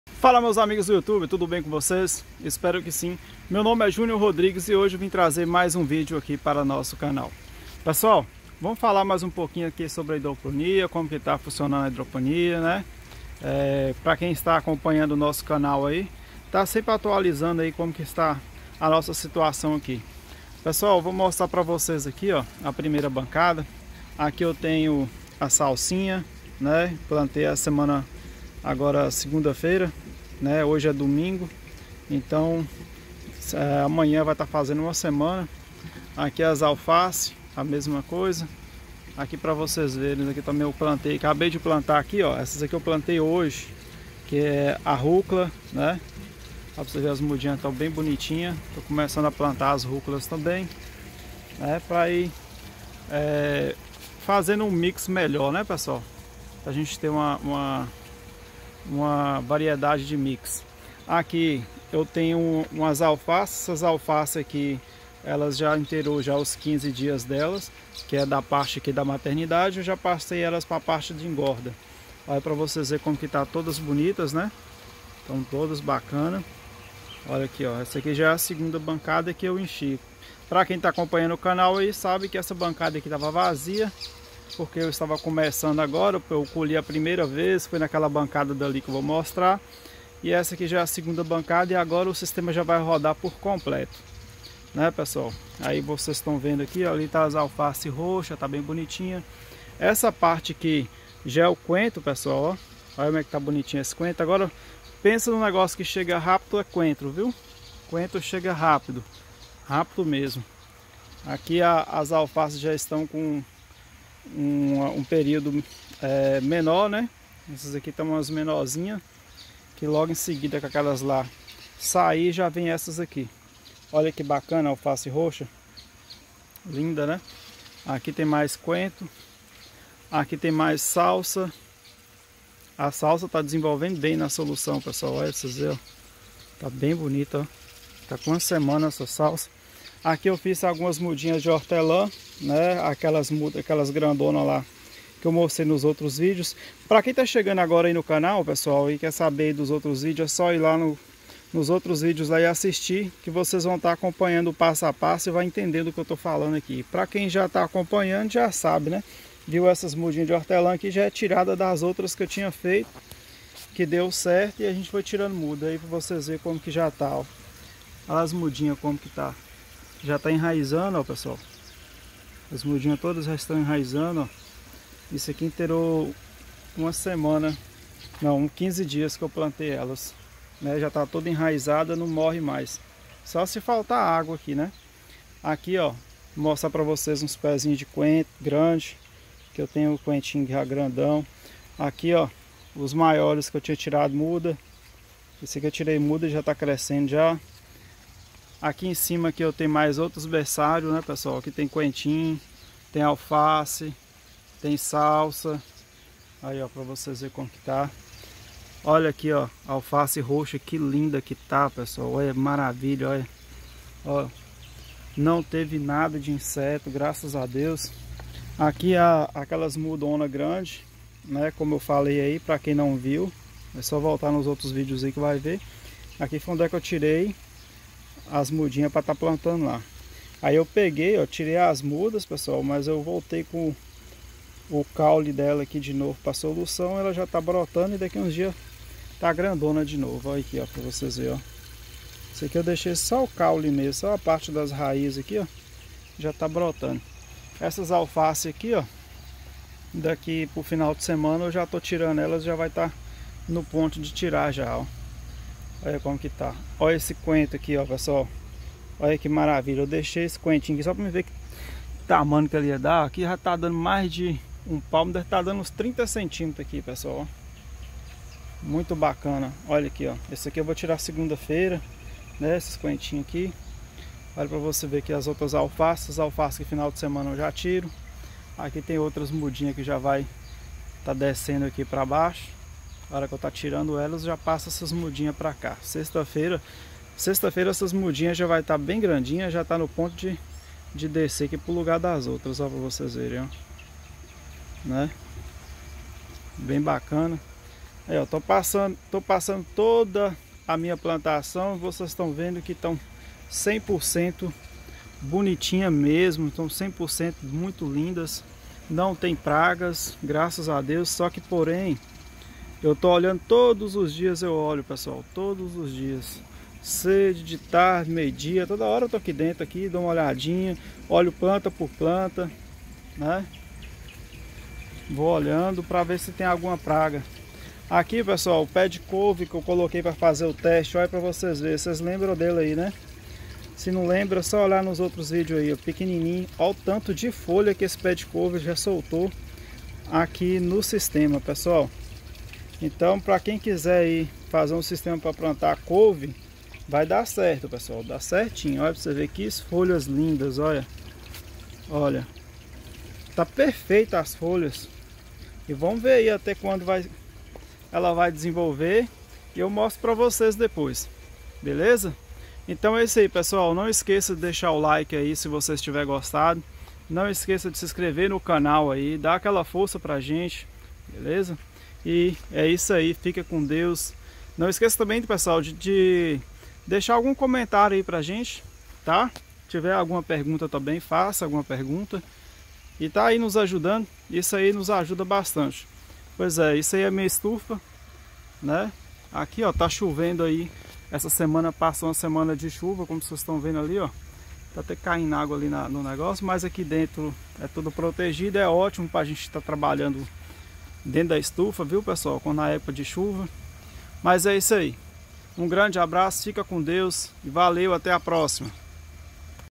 Hidroponia toda já completa. Rumo a segunda faz.
Aqui venho mostrar como está sendo o meu resultado na hidroponia.